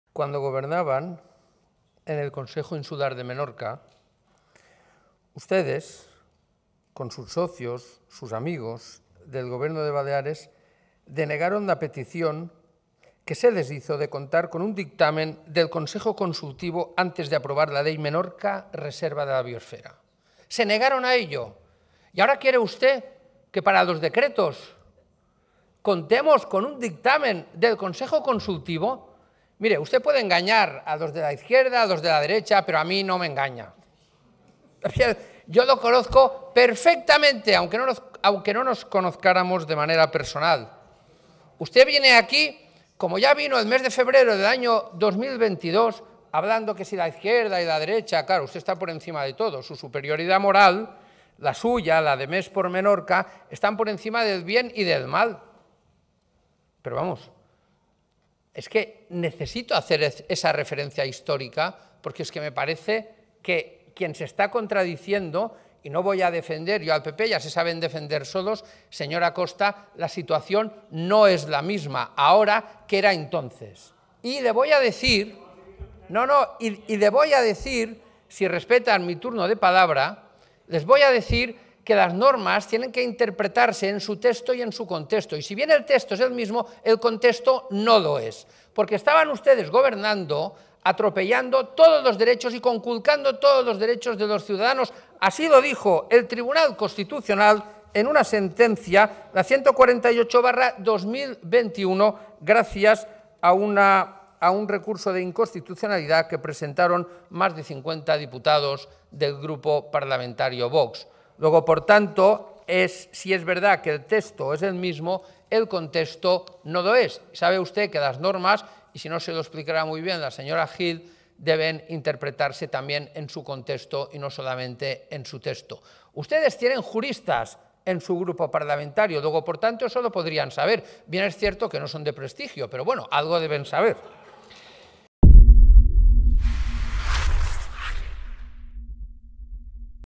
📣📣 Xisco Cardona en el Parlamento balear:
“Cuando Vds. (Més per Menorca) gobernaban con sus socios (PSOE) 👎 en el Consejo Insular de Menorca denegaron la petición que se les hizo de contar con un dictamen del Consejo Consultivo antes de aprobar la Ley Menorca Reserva de Biosfera 😡¿Ahora quiere que para los decretos contemos con un dictamen del Consejo Consultivo? ¡Vaya incoherencia! 🤦🏻‍♂️